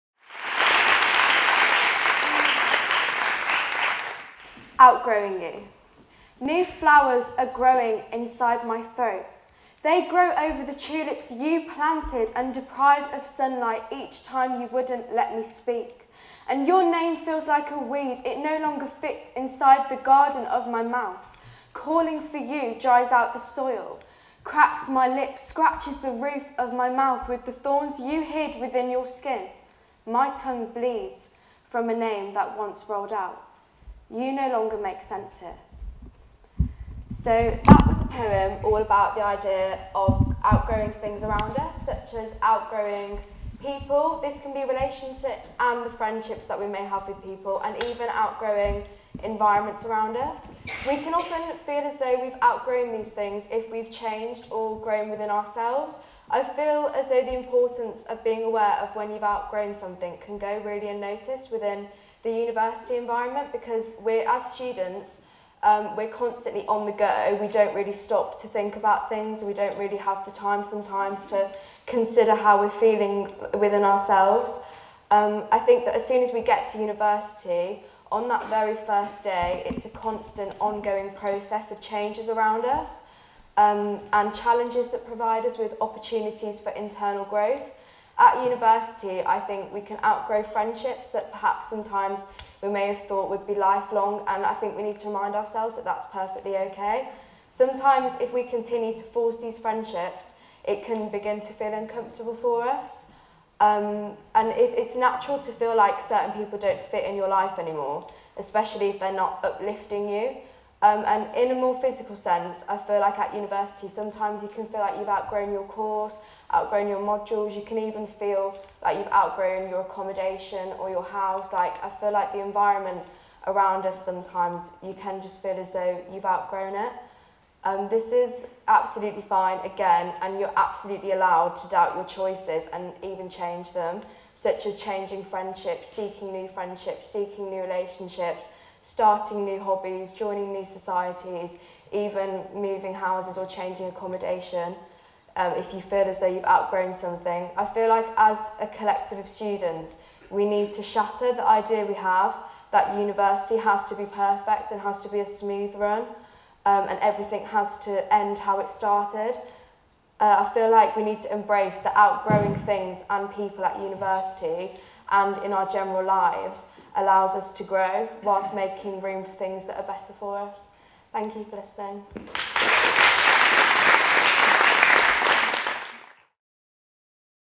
Outgrowing You | Niamh Murphy | TEDxBSU
A poem called outgrowing you- namely about outgrowing people and things around us, the importance of embracing the idea of letting go, outgrowing in general life and the environment and University.
Niamh is a 3rd Year Creative Writing student who wants to showcase her passion for poetry and spoken word, highlighting the importance of speaking your emotions. This talk was given at a TEDx event using the TED conference format but independently organized by a local community.